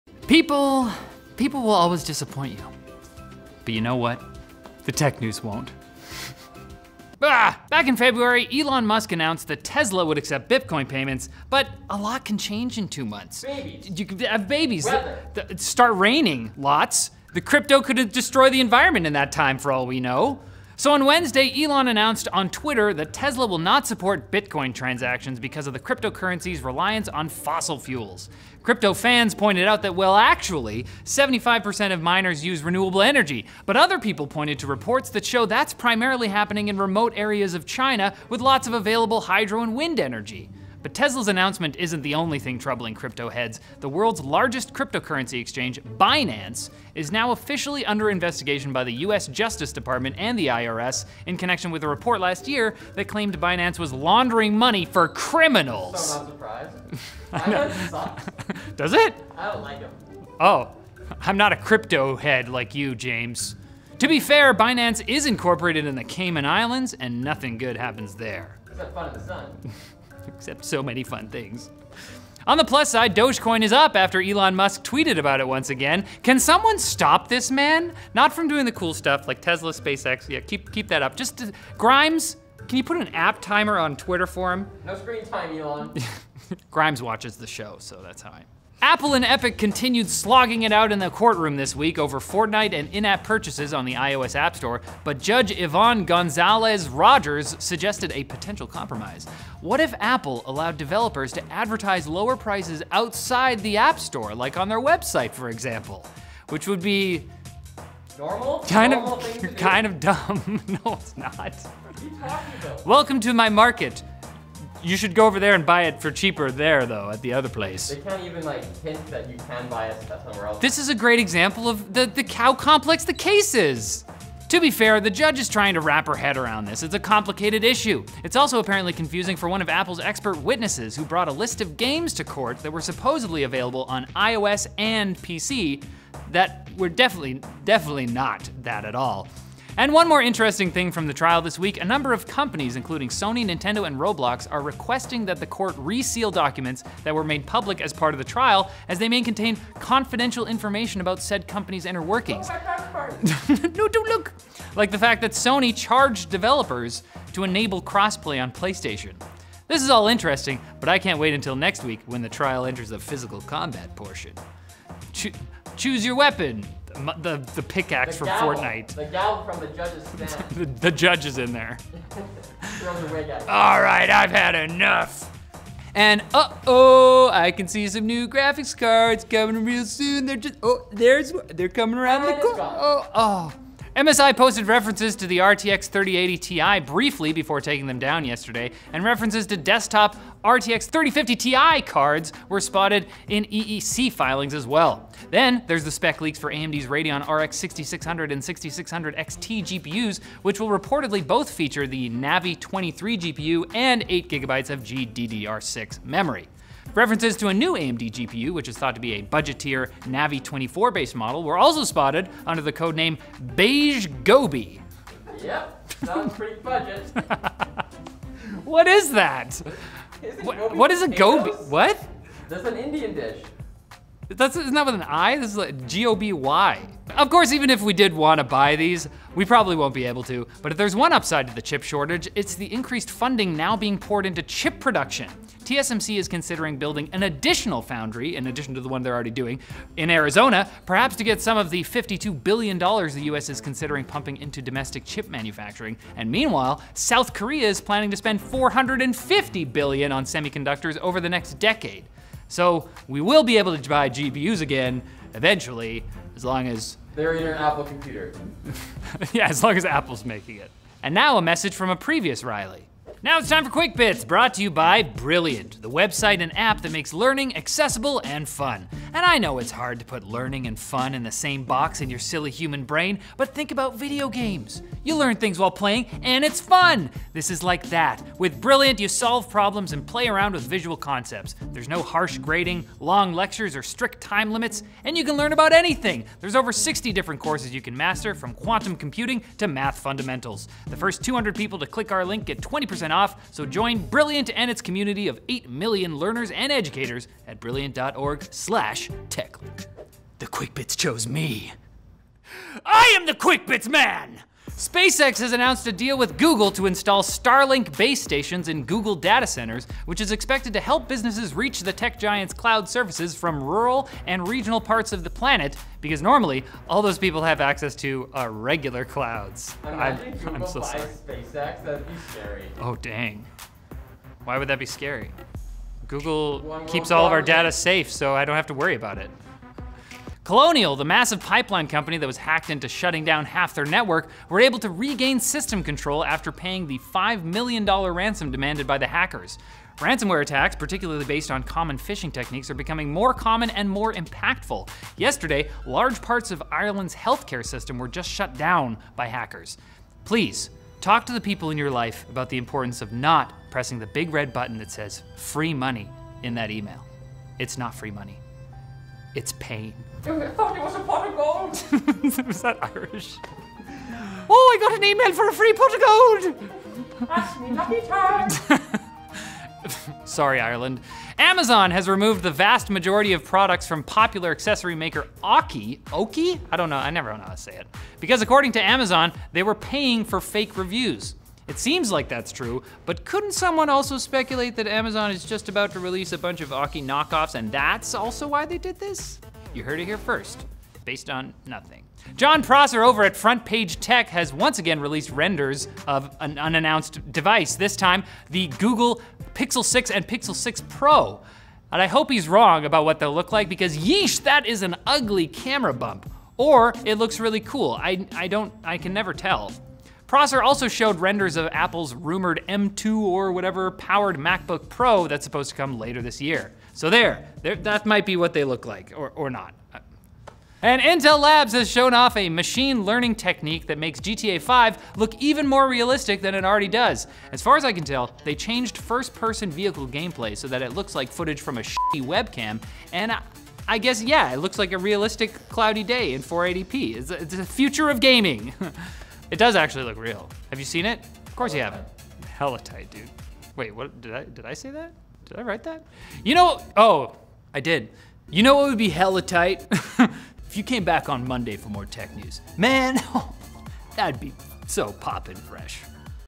Stop Tweeting, Elon!!
Timestamps:
0:00 Trust in the Tech News
0:07 Tesla drops Bitcoin, Binance investigated
1:28 Apple v Epic update
2:55 RTX 3080 Ti, RX 6600, chip shortage
4:21 Brilliant
4:59 QUICK BITS
5:04 SpaceX + Google deal
5:30 Colonial regains control
6:11 Amazon removes Aukey products
6:32 Pixel 6 Pro, Macbook Pro renders
7:00 GTA V photorealistic filter

NEWS SOURCES

BIT OFF MORE COIN THAN U CAN CHEW
Tesla suspends Bitcoin support

QUICK BITS

MAGELLANIC CLOUD STORAGE
SpaceX makes deal with Google Cloud

RE-TAKING THE COLONIES
Colonial regains control after paying $5 million

THIS IS NOT AUKAY
Amazon removes Aukey products because of fake reviews

CE N’EST PAS UN PIXEL 6
Google Pixel 6, M2 Macbook Pro renders

GRAND THEFT DASHCAM
Intel Labs makes GTA 5 photorealistic